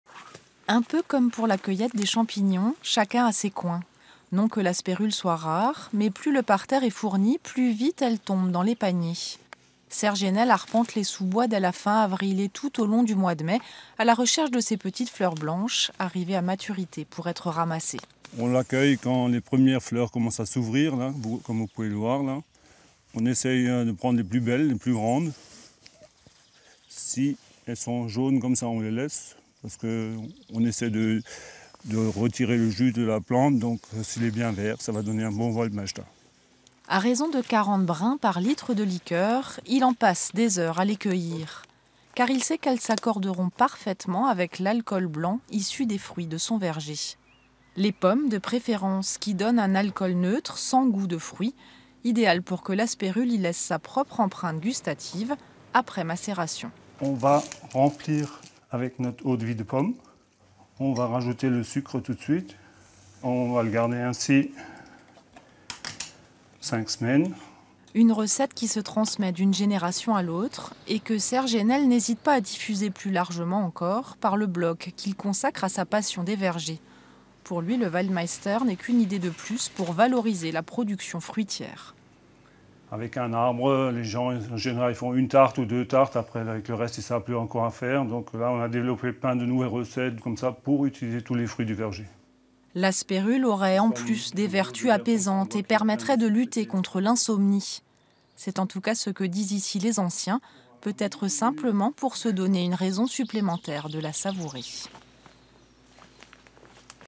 Recette liqueur d'aspérule ou Waldmeister (reportage France 3 Alsace)
Il s’agit en fait d’un macération de schnaps, sucre et d’une plante aromatique : le gaillet odorant (Asperula odorata) également appelé aspérule odorante. Retrouver toute la fabrication sur ce blog